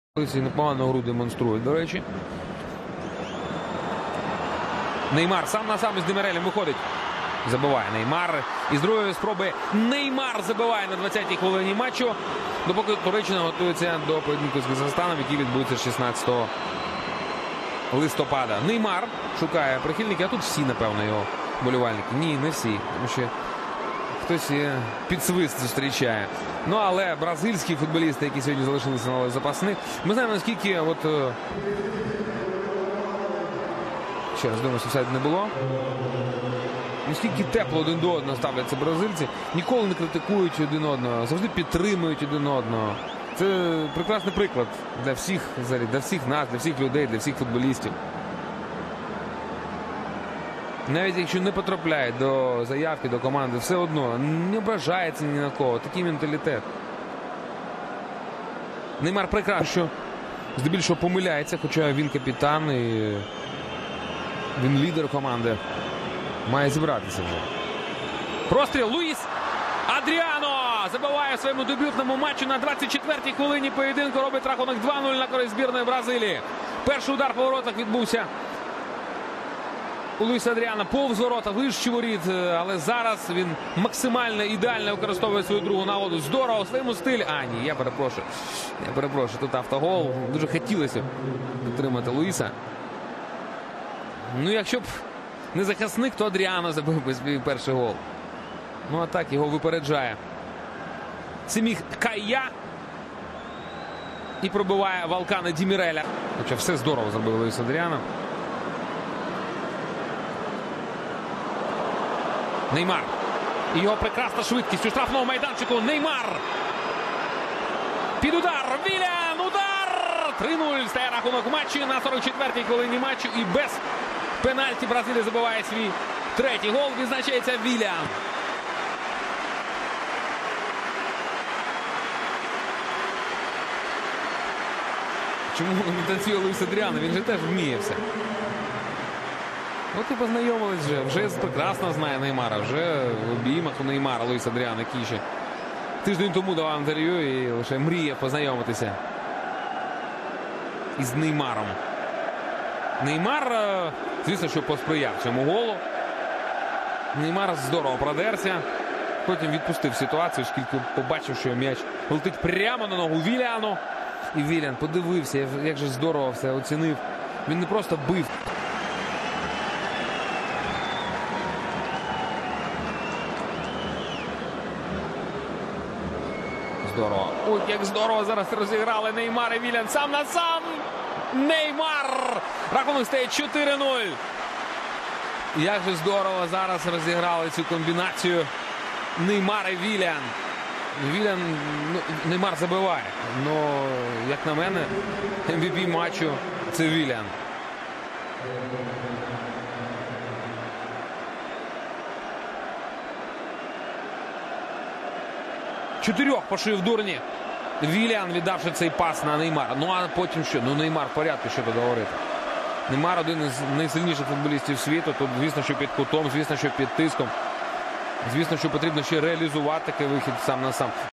Turkey 0 – 4 Brazil - Friendly - 12.11.2014
AGEN JUDI | Agen Bola | Bandar Judi | AGEN SBOBET | Judi Online

Turkey vs Brazil
Competition: Friendly
Match date: 12 November 2014
Goals: Brazil – Neymar 20, Semih Kaya (og) 24, Willian 44, Neymar 60;
Venue: Ataturk Olimpiyat Stadi, Istanbul